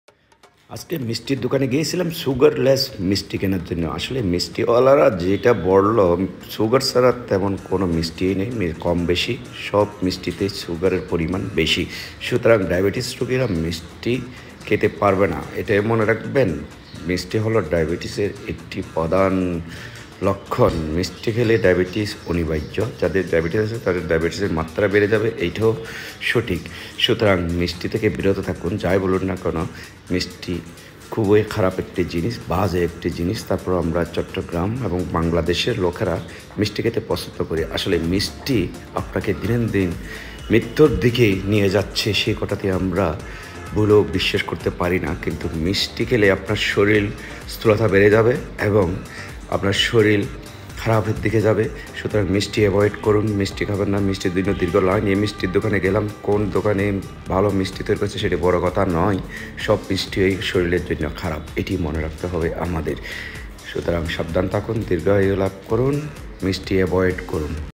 সুস্থ থাকুন। মিষ্টিজাতীয় খাবার পরিহার করুন।
সুস্থ থাকুন' মিষ্টিজাতীয় খাবার এভোয়েড করুন। মিস্টি খেলে শরীরে ডায়াবেটিস অনিবার্য।